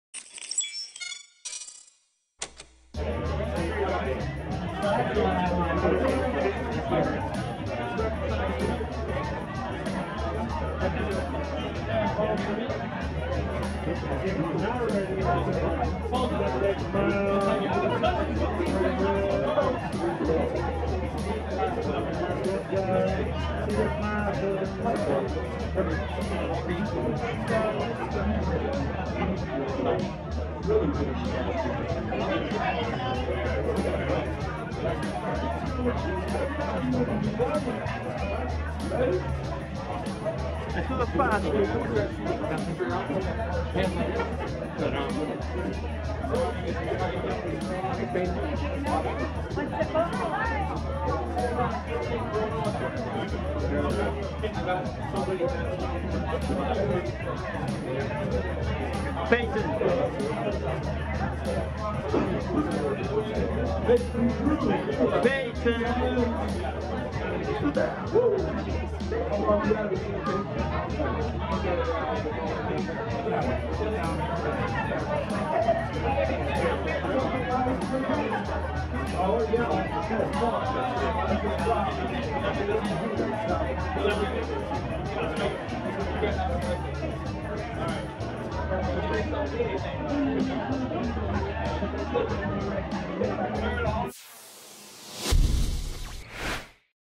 Peyton Drew 2019 Babes in Toyland LA Toy Drive Red Carpet Fashion
Exclusive broll footage: Peyton Drew ❥♛ @peyton.drew on the red carpet at the 12th Annual "Babes in Toyland: Christmas Toy Drive" charity event benefiting The Midnight Mission held at the Avalon Hollywood in Los Angeles, California USA on December 4, 2019